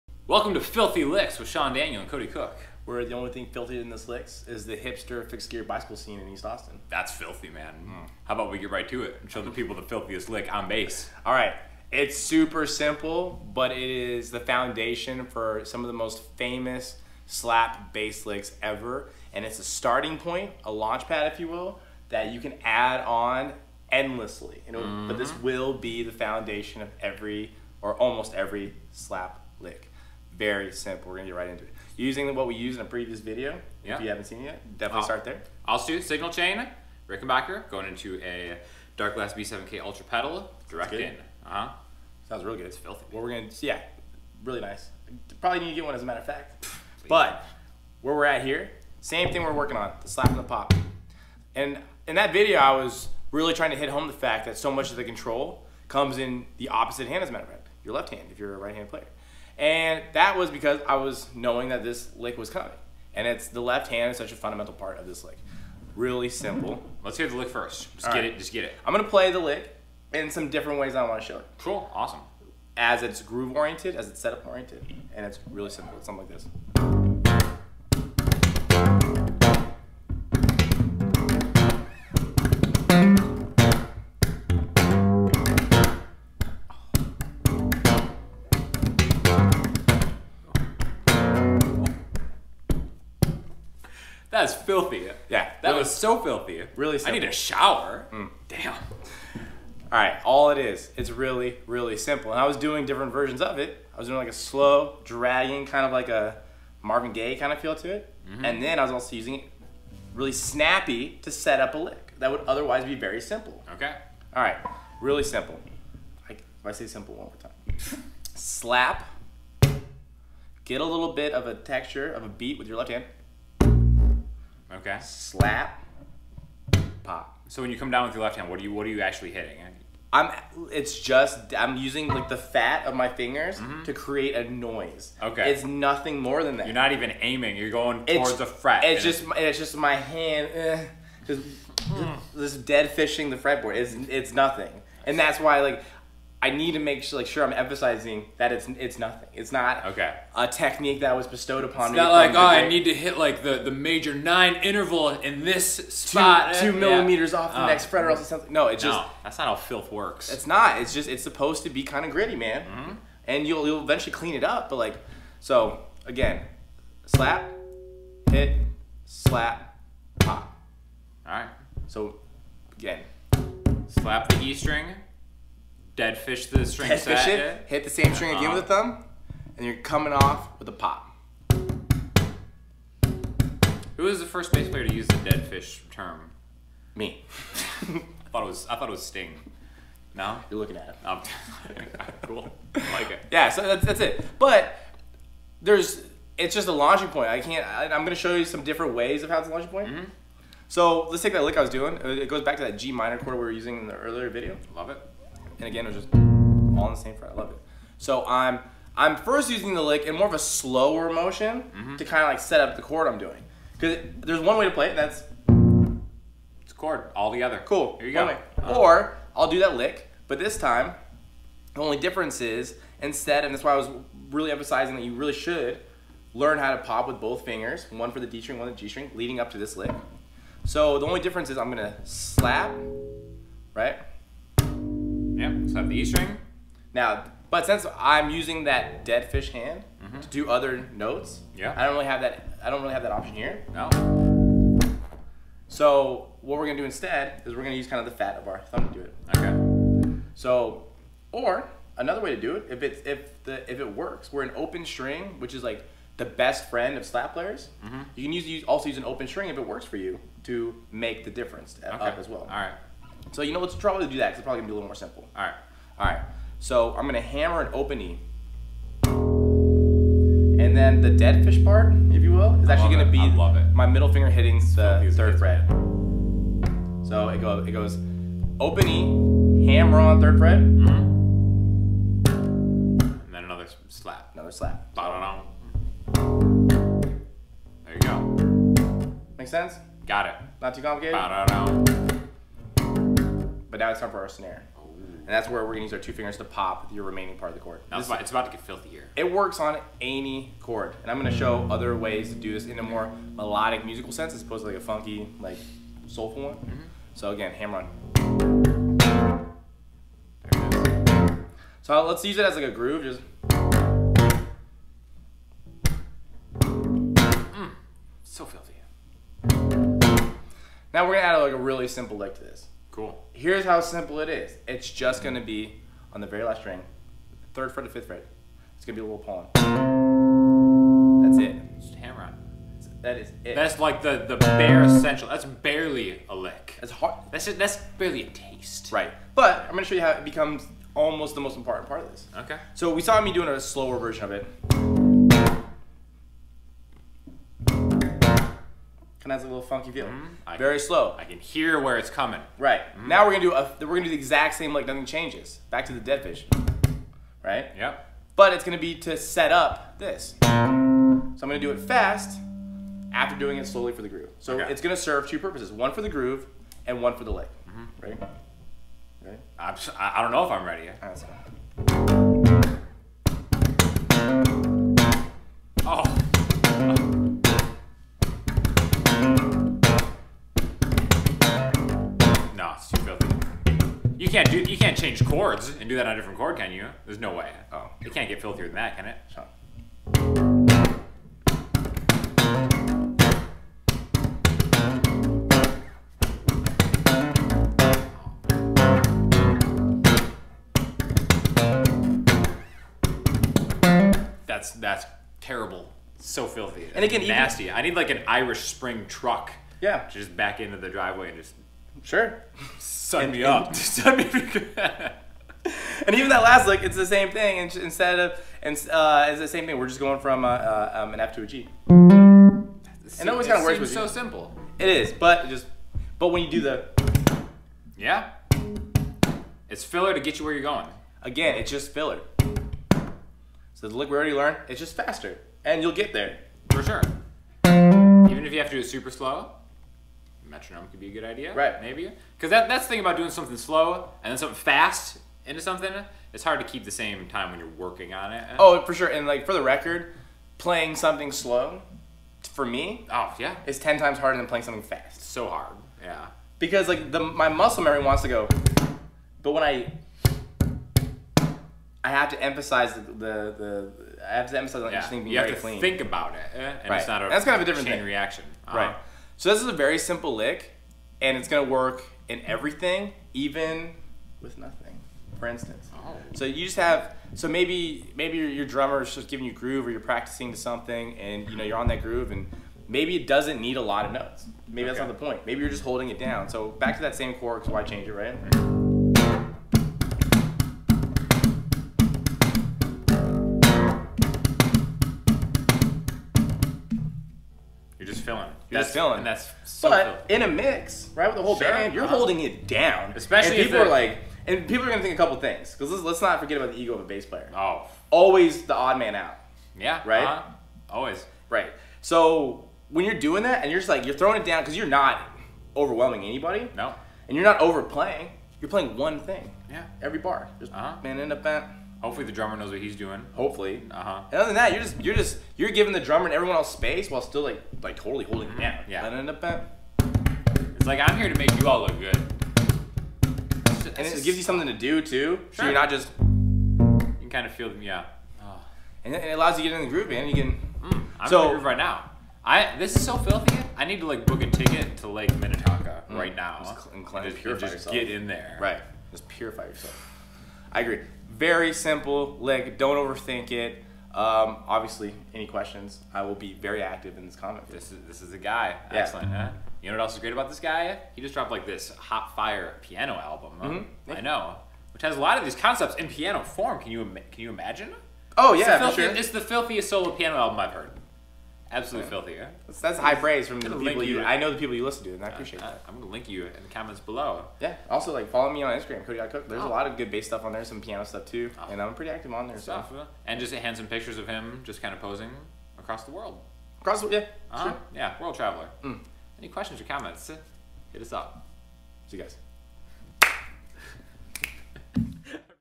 How to Play a Filthy Bass Lick
Back with my guy Cody Cook to teach some nasty bass licks and rhythms. Here's the first bass video we did if you missed it

Make sure to head on over to his channel for more lessons and music and make sure to show him some love!